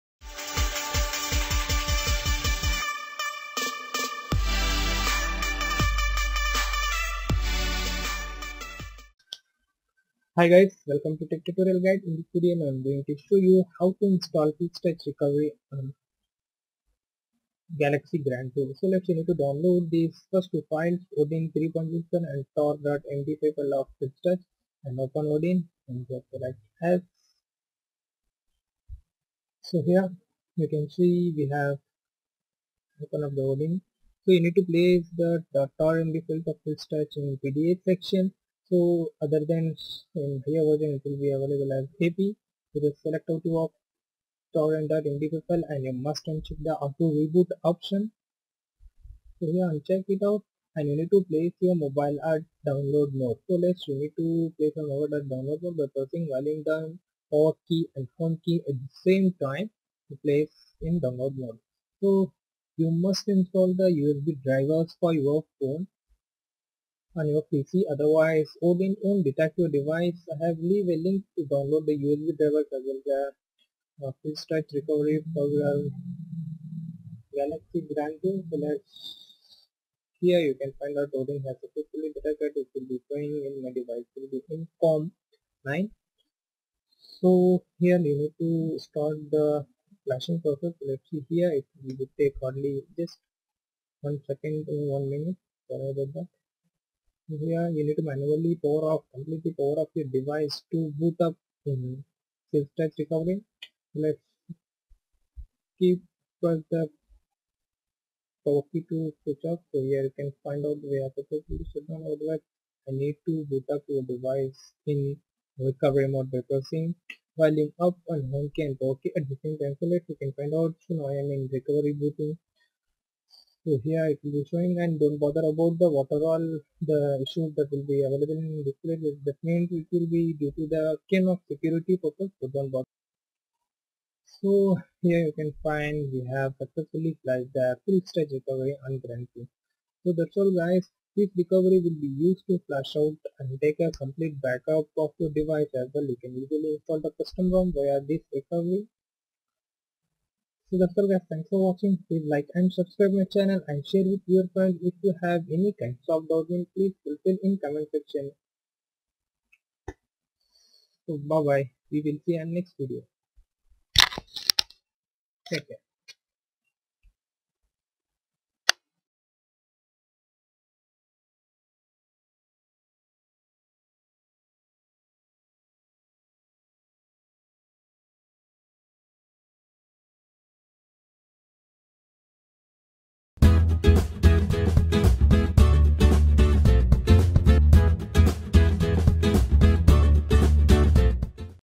Install Philz Touch Recovery On Galaxy Grand 2
How  To Install Philz Touch Recovery On Galaxy Grand 2  [G7102]!

In This Video as i have shown step by step guide to install philz touch recovery on grand 2.

*-Philz Recovery Is Nothing But Its A kind Of Custom Recovery Which Having Lots Features While comparing With Stock Recovery.

*-You Can Easily Take A Full BackUp Of Your System.

*-You Can Flash Rom Using This Recovery..

Grand 2 Philz Touch [.tar] = 2 philz

TechTutorialguide